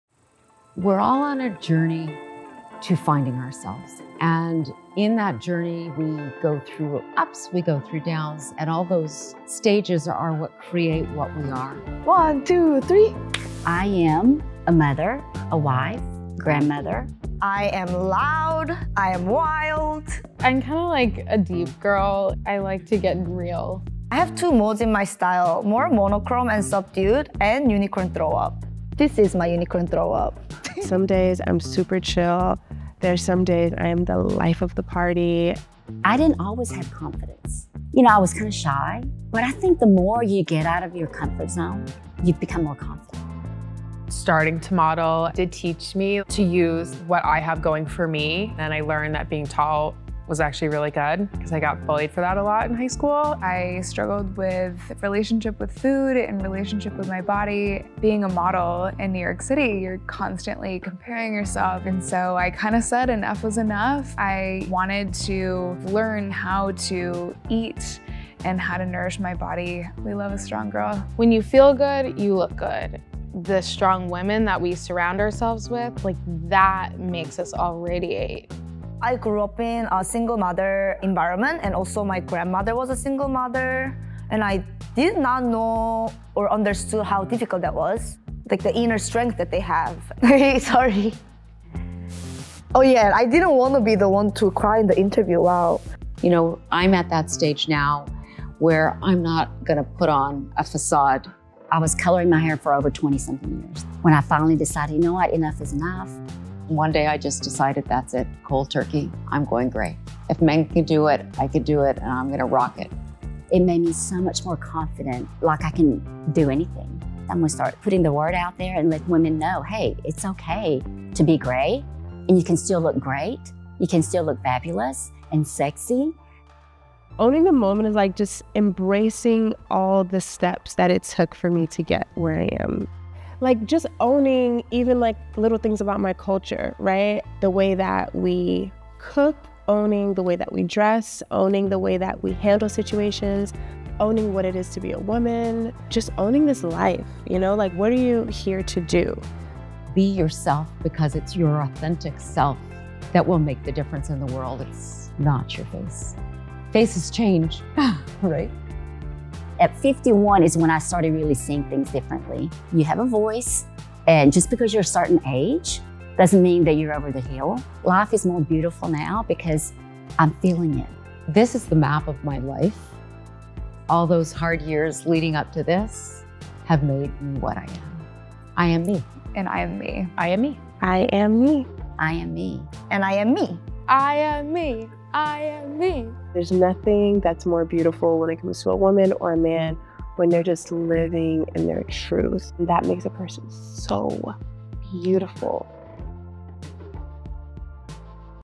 Introducing The I Am Me Campaign By Jones Road Beauty
Jones Road Beauty’s I Am Me campaign supports knowing who you are and owning it.

It’s about consciously focusing on what you like about yourself, and embracing what makes you unique. The campaign promotes freeing yourself from impossible standards and encourages a kinder shift in the narrative of how we speak to ourselves. I Am Me highlights women from all backgrounds, sharing their individual journeys to finding confidence and true happiness in their lives and in their beauty.